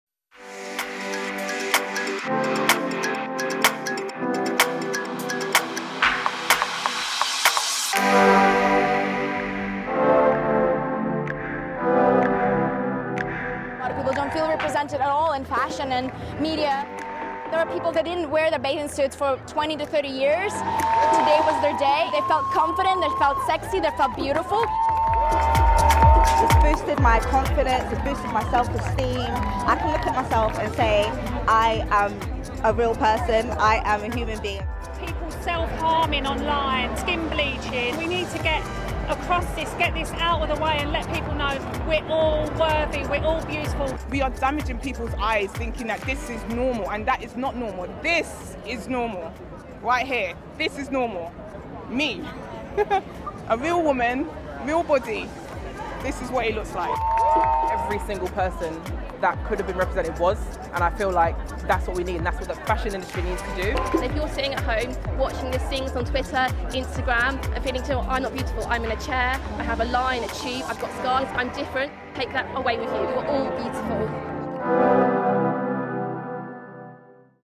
Ҳақиқий гўзаллик нимада? Моделлар баданидами?..- BBC Uzbek
Лондондаги моделлар супачасини оддий одамлар ишғол қилишди. Гўзаллик ҳақида барча қолип тушунчаларни "вайрон" этишди.